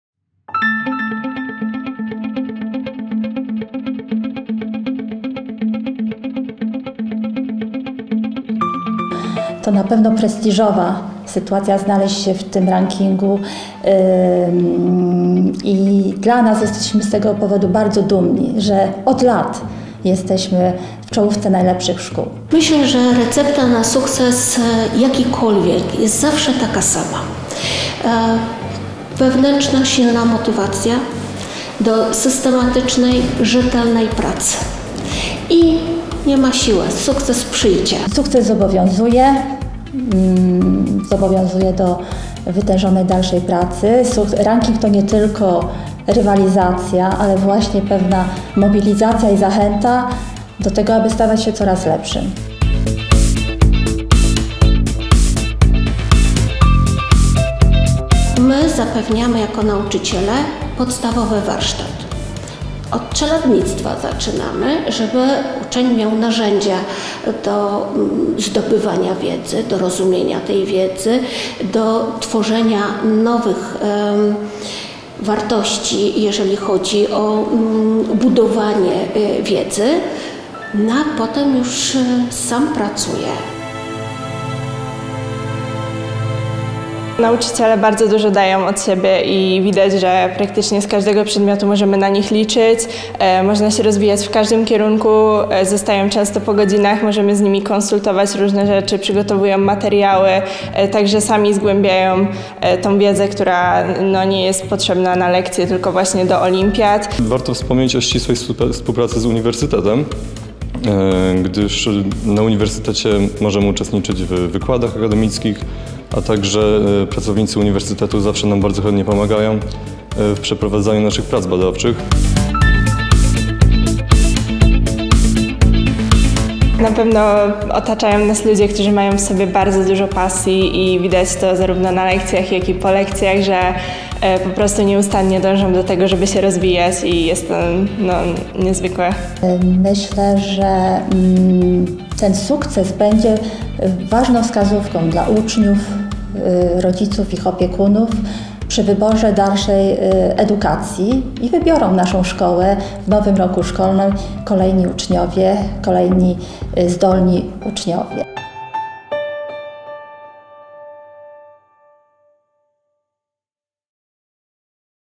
Mamy najlepsze liceum w Polsce
Uniwersyteckie Liceum Ogólnokształcące znów najlepsze w Polsce! Swój doroczny ranking szkół ogłosił miesięcznik "Perspektywy".

Ranking "Perspektyw", obejmujący półtora tysiąca polskich liceów i techników, powstaje na podstawie wyników matur oraz sukcesów uczniów w olimpiadach przedmiotowych. O jego ostatecznym kształcie decyduje kapituła, na której czele stoi prof. dr hab. Jan Łaszczyk, były rektor Akademii Pedagogiki Specjalnej i przewodniczący Komisji ds. Kształcenia KRASP.